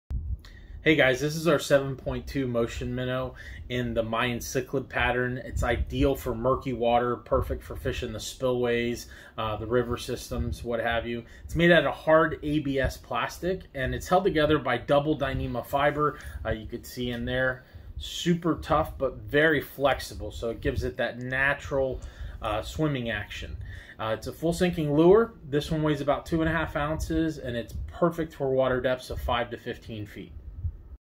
7.2" Mayan Cichlid Motion Minnow Swimbait Lure
Our Motion Minnow 7.2 Inch Mayan Cichlid Segmented Swim-Bait Fishing Lure was built for fresh and brackish water anglers that demand a larger profile for trophy Largemouth Bass, Freshwater Dorado, Snook, Peacock Bass, Tarpon, and MANY other gamefish.

Our Mayan Cichlid segmented fishing lure mimics a "candy bar" size Cichlid which trophy predators inhale throughout Florida and Central America.  Held together with a double matted Dyneema® Fiber, a material that is 15x stronger than steel and rivals kevlar. Sometimes its hard to find live bait, so we created the next best thing!